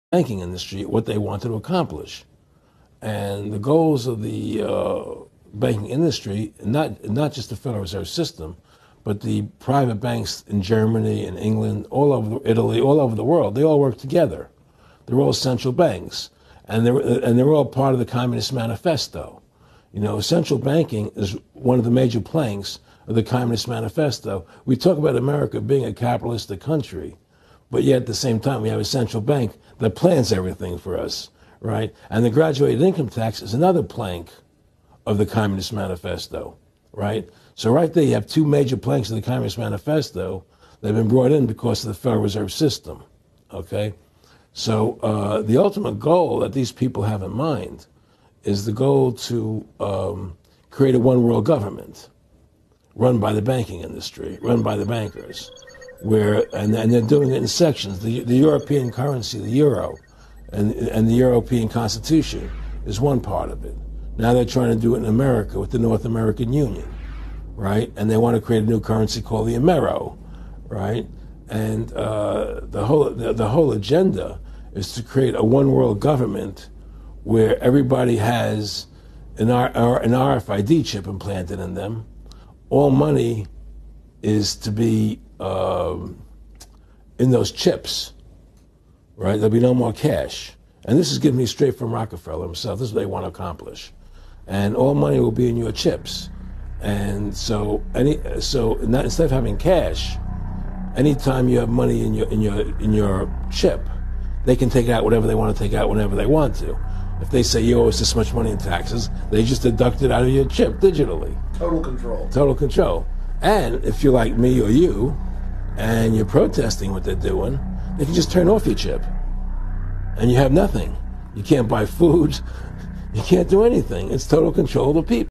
Aaron Russo, explains the core of what the "Corona Virus"
Rockefeller insider, Aaron Russo, explains the core of what the "Corona Virus"/New World Order Agenda in 2 minutes!